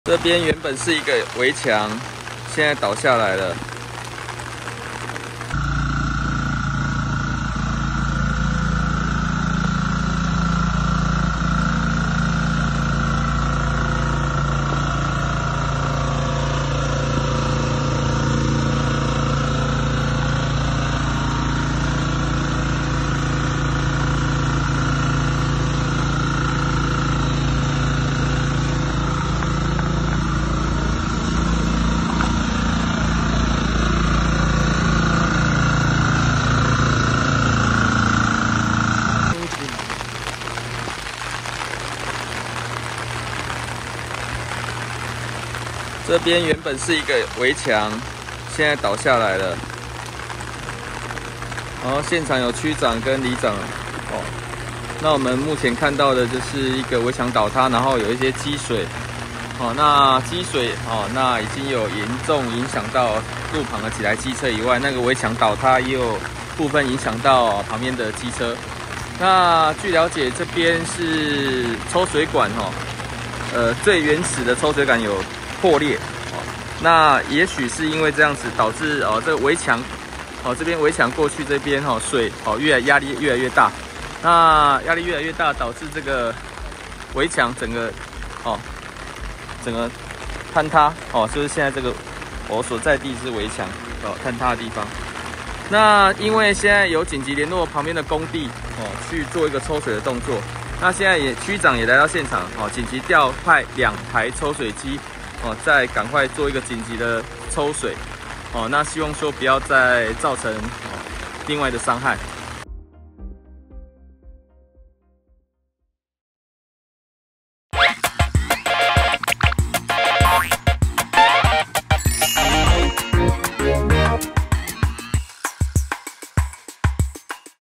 汐止崇德國小旁圍牆倒塌 數輛機車慘遭波及畫面曝
【相關報導】
快訊／全台豪大雨不斷！　汐止崇德國小積水巷弄圍牆倒塌